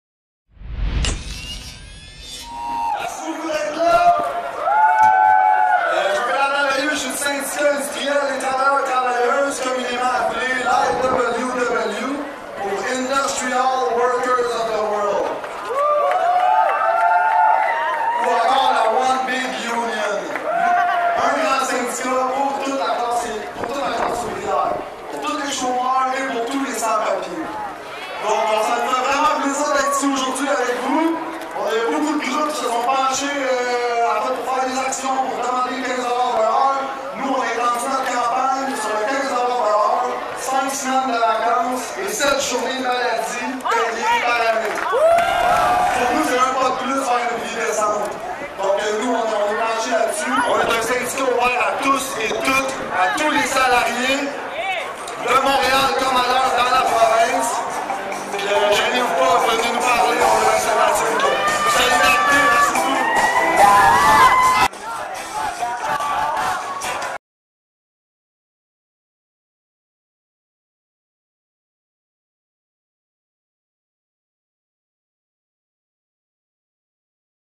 99% Media - 15 Avril 15$ - Manifestation pour le salaire minimum de 15 dollar
15 Avril 15$. Manifestation pour le salaire minimum de 15 dollar de l’heure et pour une vie décente